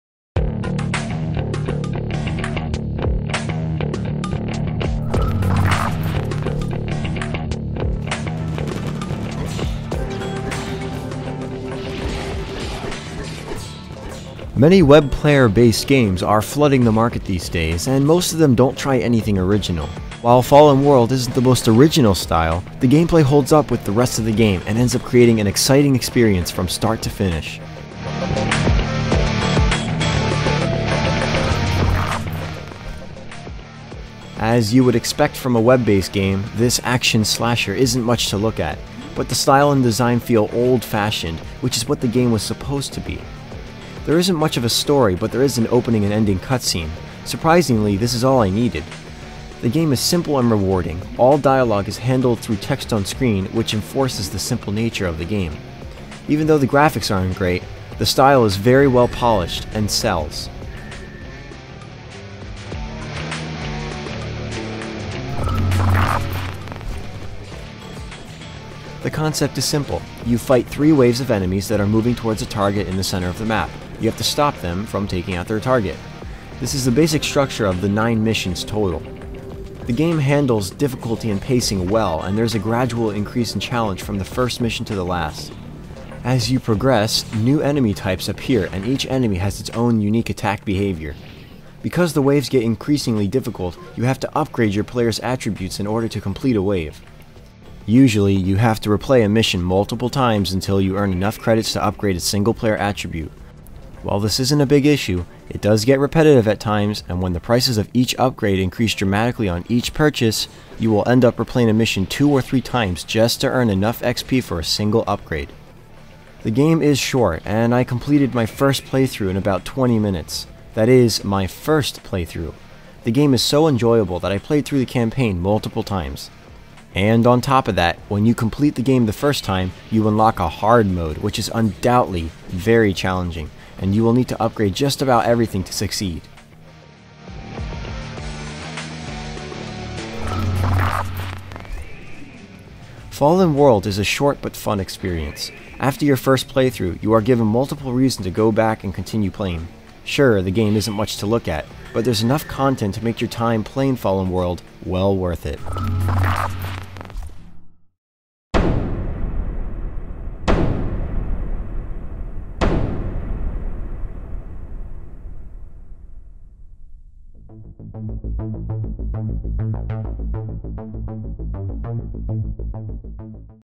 GAME REVIEW - Fallen World
Is Fallen World just another action slasher, or does it bring something new to the genre?